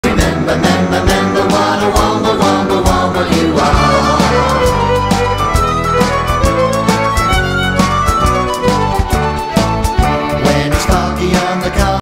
Scottish Terrier Shadow Wombling Around 👀
Shadow does his Wombling bit, checking out all the rubbish others have left behind on the fields 🙄